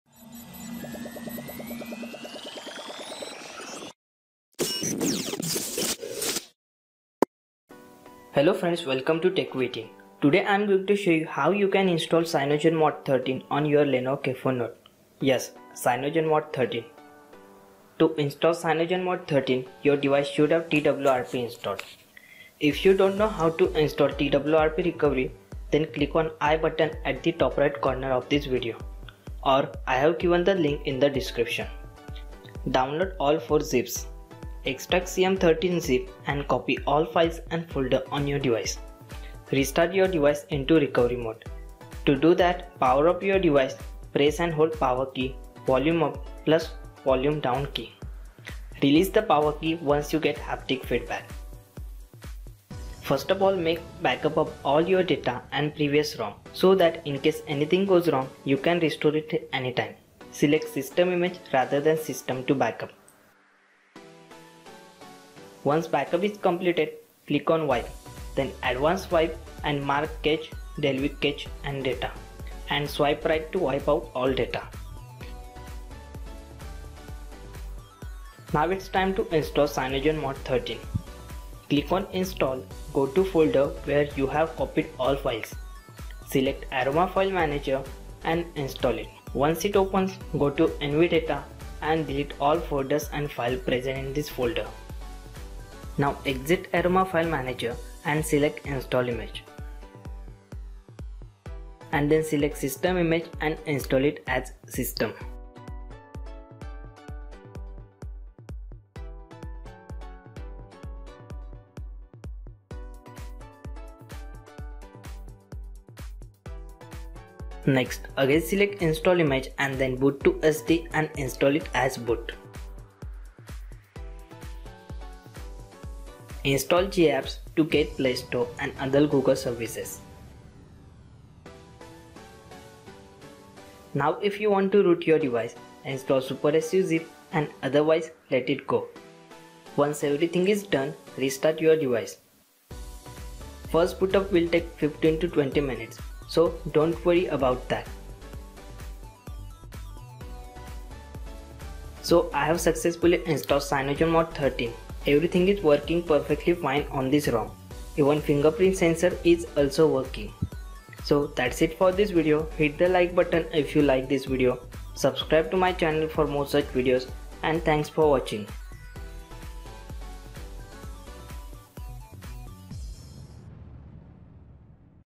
Install CyanogenMod 13 on Lenovo K4 note/A7010/Vibe X3 lite
Wait is over. Now you can install CM13 on your lenovo k4 note. Follow all steps properly.
Fingerprint sensor is also working in this ROM.

All credits to Zormax for this ROM.

Note : Your device should have TWRP (3.0.2 or above version) installed.

Bugs :

1. No LTE/4G/3G symbol when roaming
2. Only one LED glows when we turn on flashlight
3. Same LED issue with Camera.
4. Rotation issue in camera app (can be solved using third party camera app)
5. NFC
6. Step counter doesn't work.

*NO MAJOR BUGS*